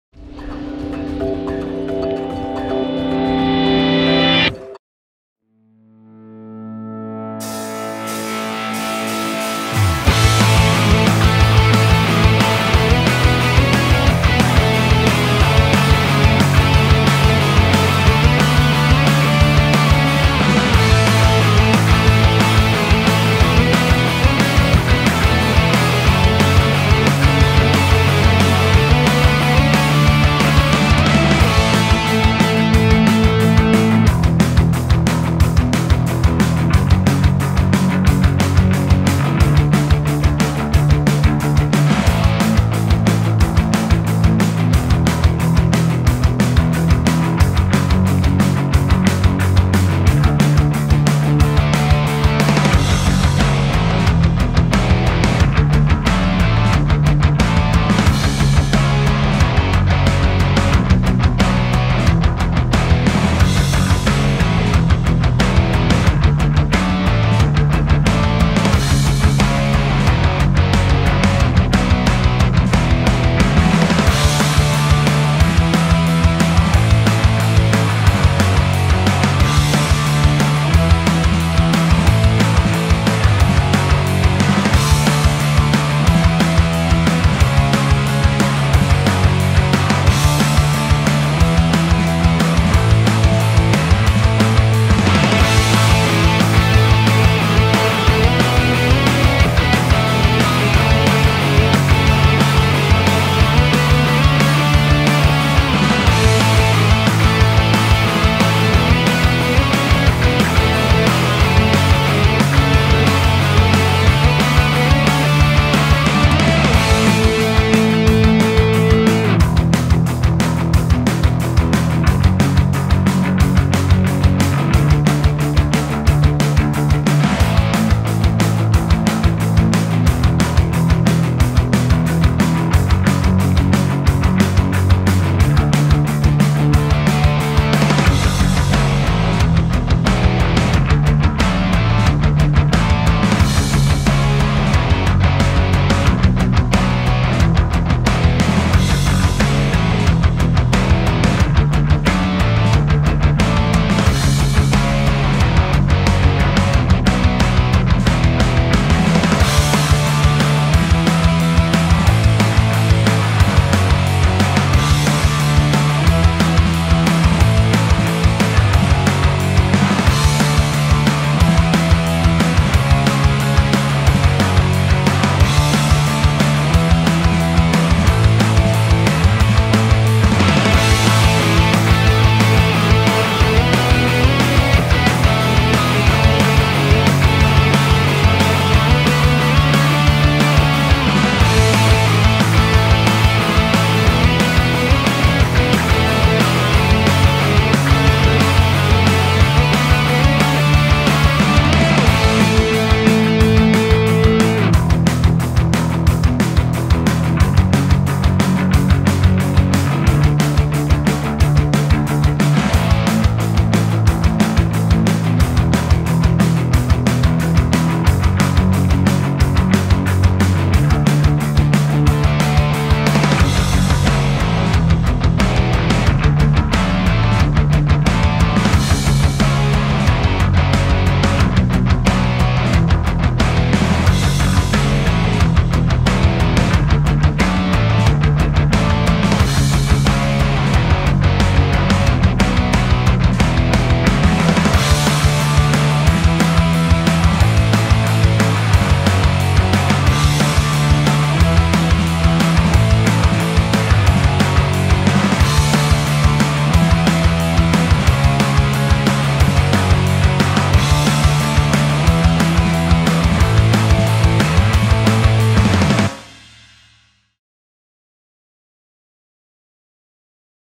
Melodic Pop Punk Backing Track in A major
Trap/Chill/Ambient/House etc... with Guitar track

KEY=A major
BPM=180